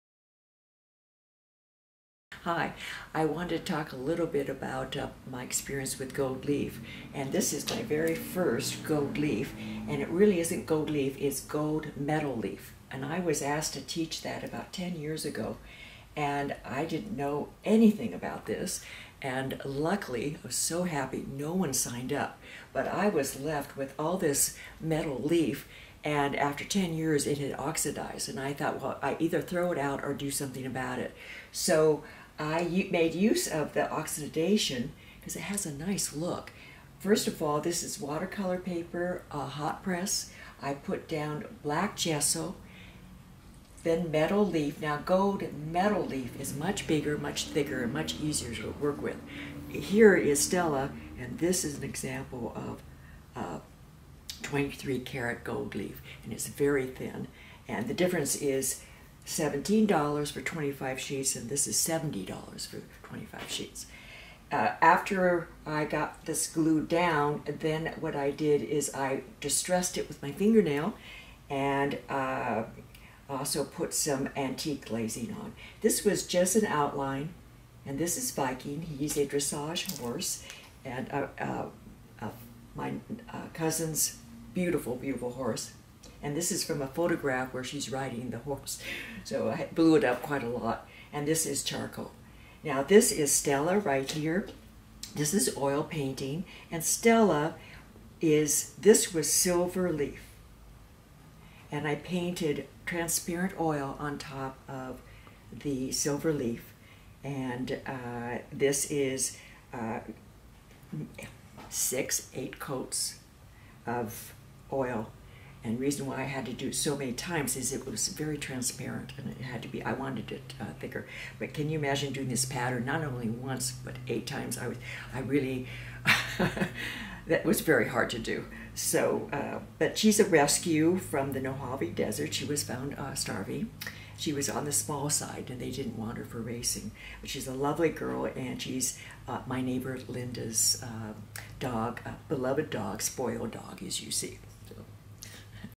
Gold Leaf Artwork Introduction With Morgan Kari
Harmony Color's introduction to Gold Leaf Artwork with fine artist Morgan Kari. In this art lesson, Morgan Kari introduces her Gold Leaf artwork including an art piece she produced with Gold Metal Art.

What is Gold Leaf Artwork?

Gold Leaf Artwork begins when gold is hammered into thin sheets and the fine artist utilizes these gold sheets to create beautiful, shimmering surfaces. This is called Gold Beating.

Harmony Color provides high quality giclee printing services for fine artists locally in Los Angeles and internationally.